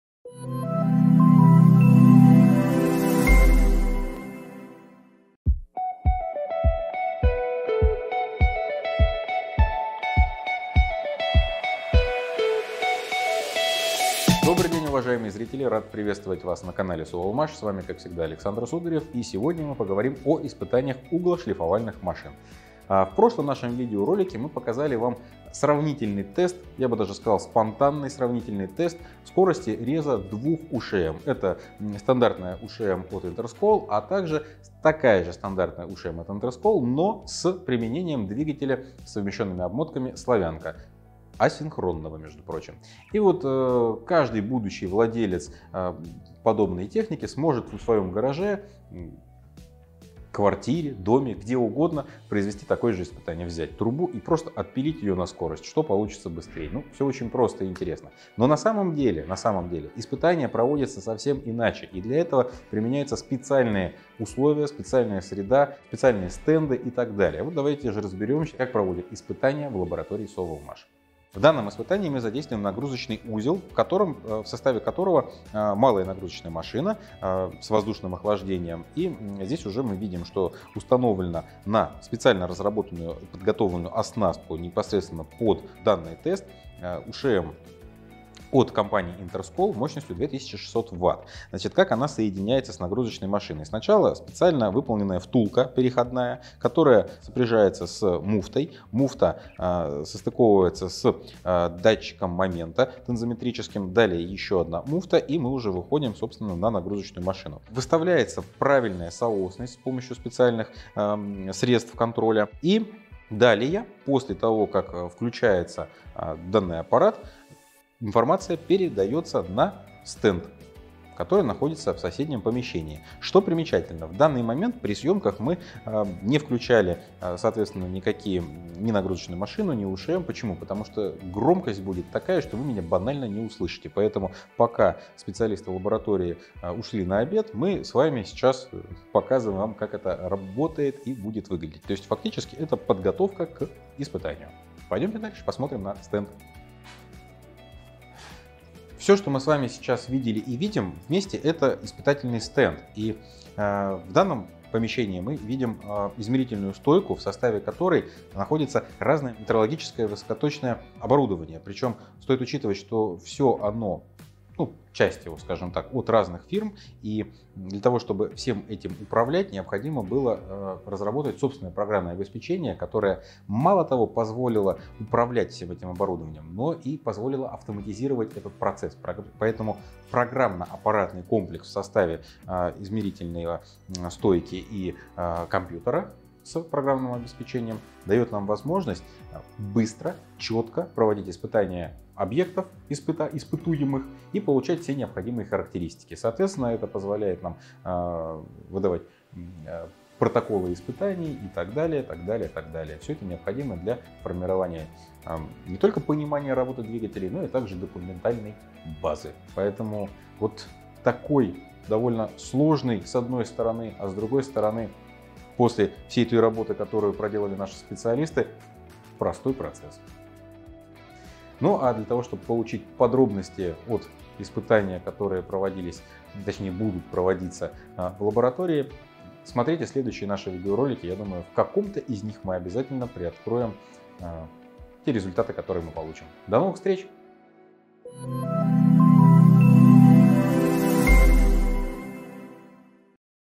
Как проводятся испытания углошлифовальных машин
Недавно мы публиковали видеоролик, в котором продемонстрировали скорость резки двух углошлифовальных машин (УШМ): стандартной от компании «ИНТЕРСКОЛ» с коллекторным двигателем и такой же от «ИНТЕРСКОЛ», но с заменённым электродвигателем разработки компании «Совэлмаш» с применением технологии совмещённых обмоток «Славянка».

В новом видеоролике  руководитель информационно-аналитической службы Александр Сударев расскажет о технологии проведения испытаний углошлифовальных машин.
Испытания УШМ проводятся на испытательном стенде с помощью программно-аппаратного комплекса, применяется специально разработанная оснастка, УШМ подключается к малой нагрузочной машине с воздушным охлаждением, применяются специальные муфты, подключается тензометрический датчик и далее информация передаётся на измерительную стойку, откуда поступает на экран монитора компьютера посредством специально разработанного специалистами «Совэлмаш» программного обеспечения.

Как выглядит оборудование и инструменты для проведения испытаний УШМ вы узнаете, посмотрев данный видеоролик!